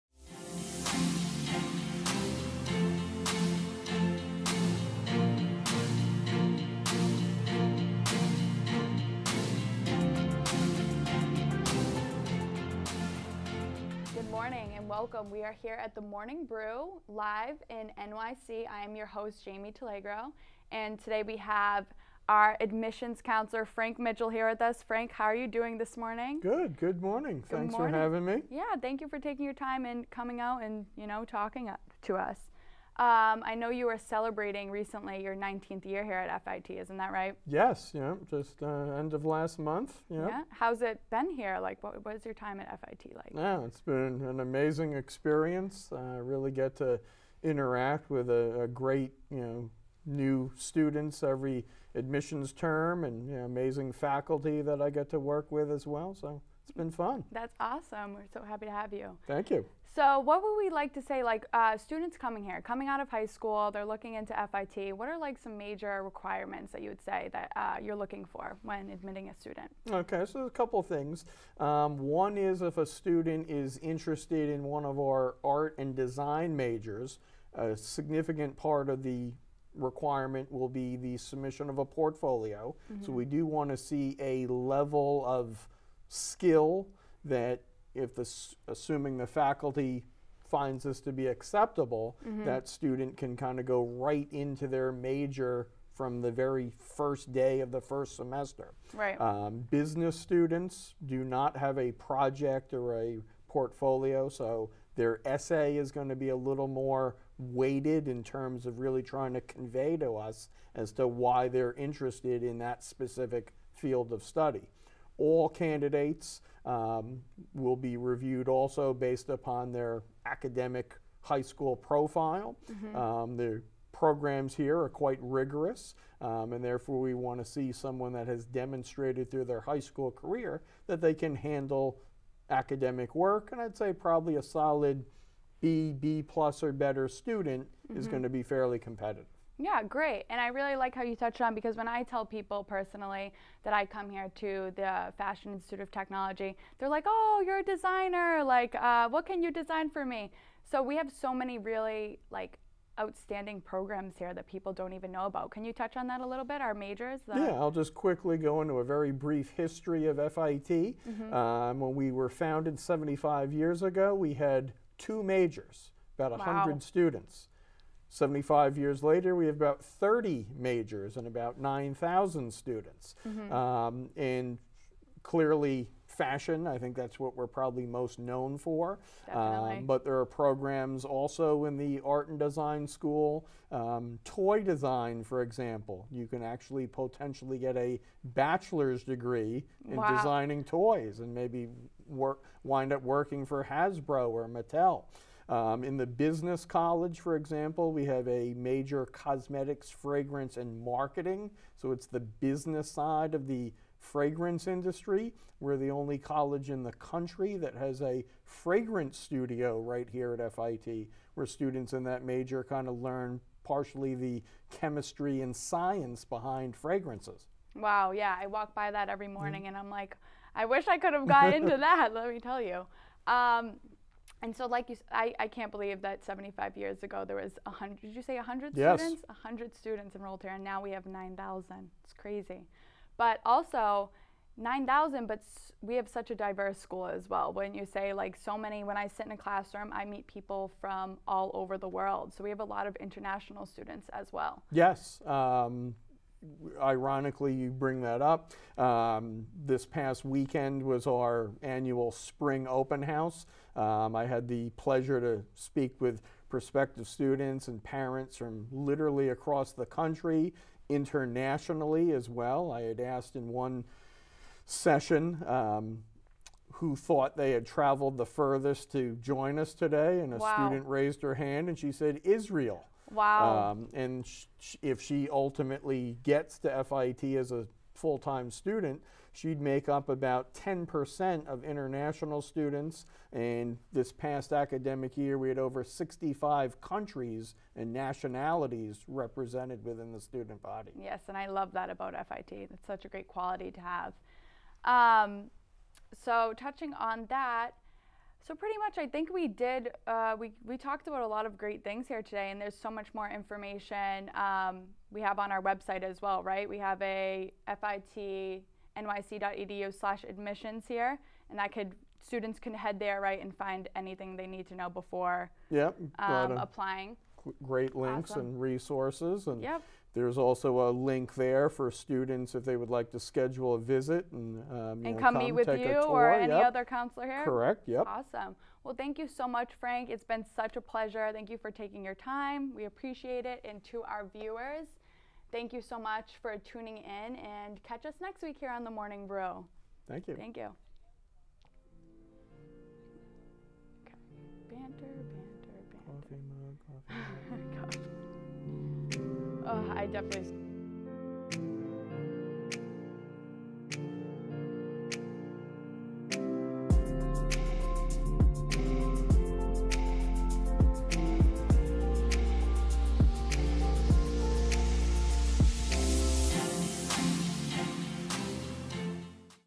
Frank Mitchell on FIT Admissions
FIT ADMISSIONS COUNSELOR FRANK MITCHELL DISCUSSES THE PROCESS OF EMBARKING ON A SUCCESSFUL CAREER AT FIT